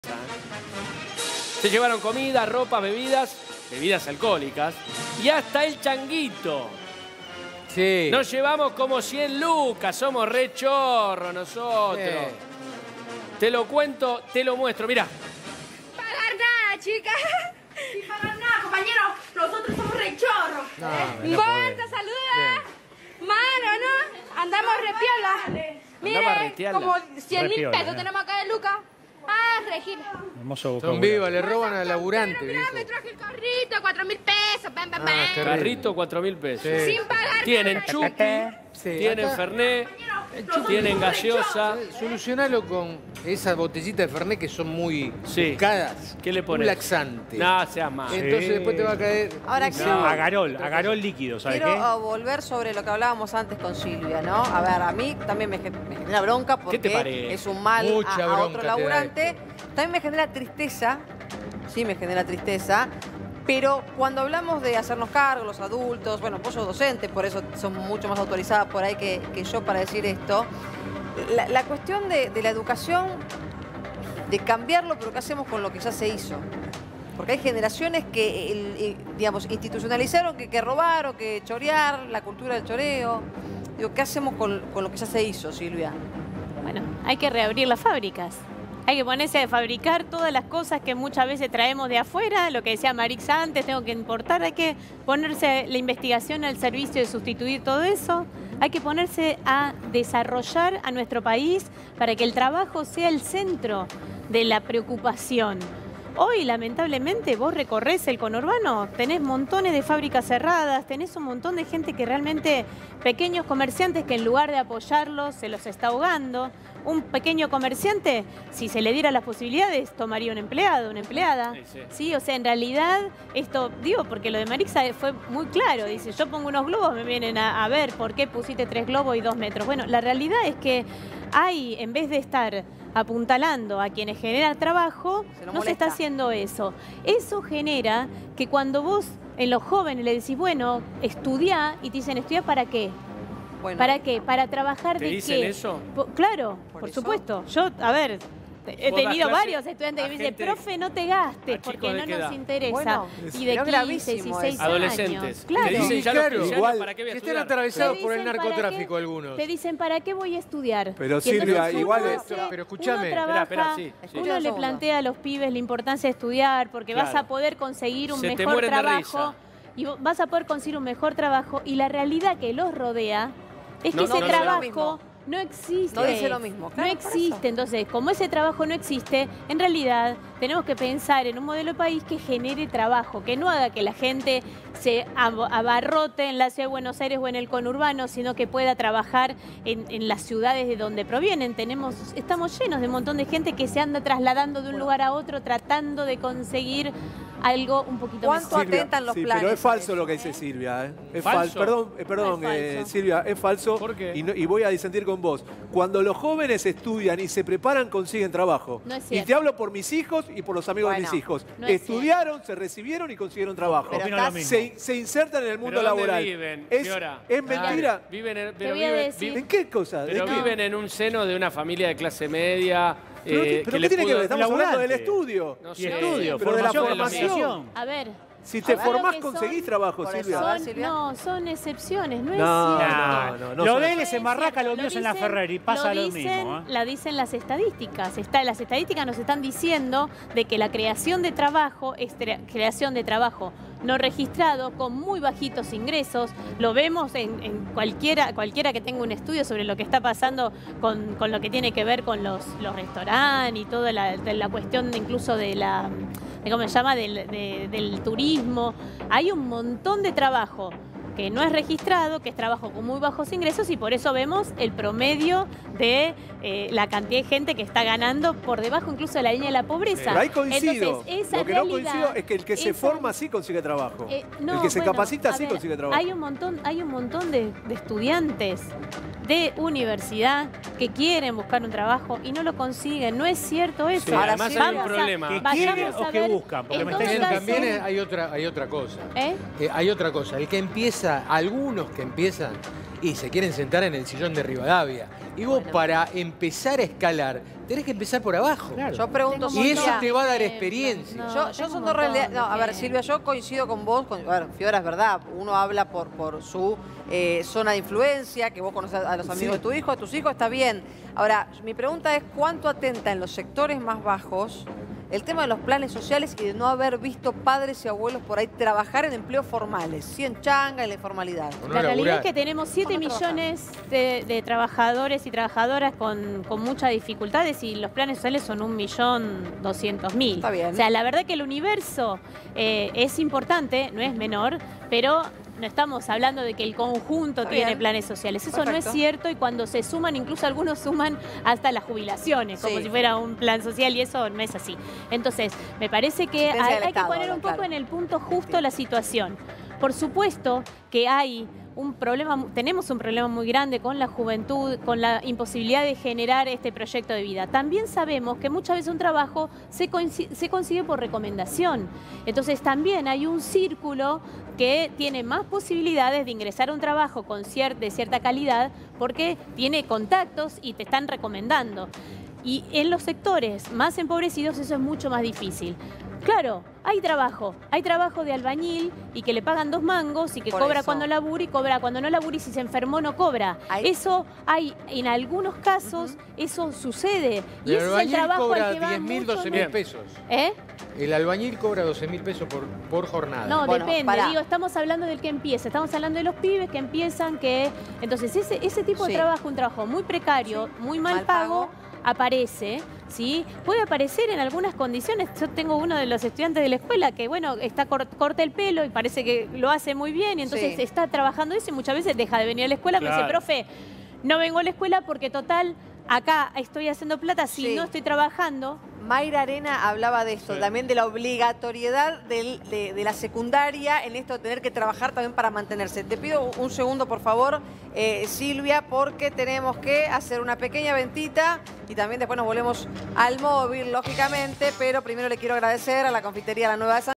Robos pirañas: así celebran los delincuentes luego de los saqueos
Luego de que varios grupos armados intentaron ingresar por la fuerza y saquear la mercadería de varios locales que estaban abiertos en la zona baja de Flores, Crónica HD te muestra cómo los delincuentes celebraron después de vaciar los negocios.

Mirá la nota.